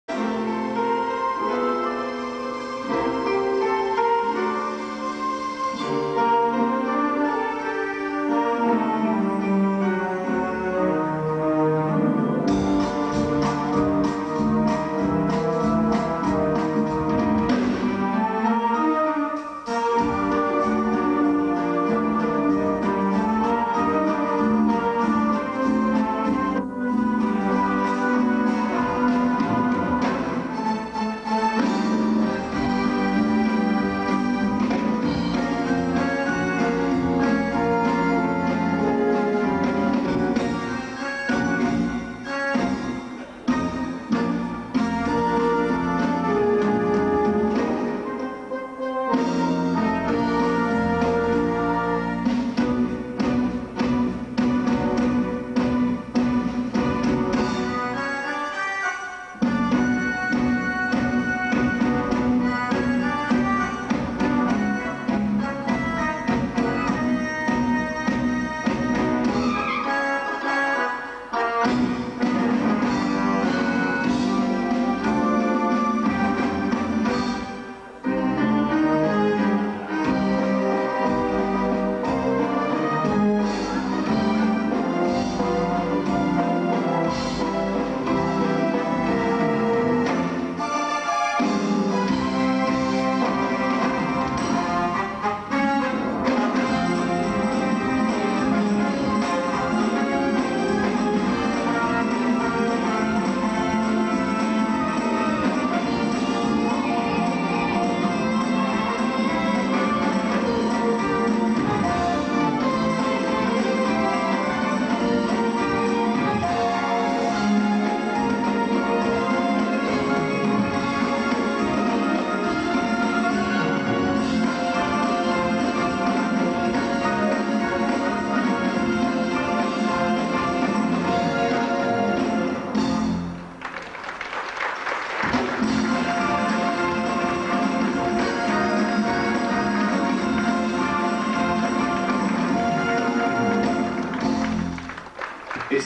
Billy Pearce comedy ballroom routine
Billy Pearce at Blackpool grand theatre doing his comedy ballroom routine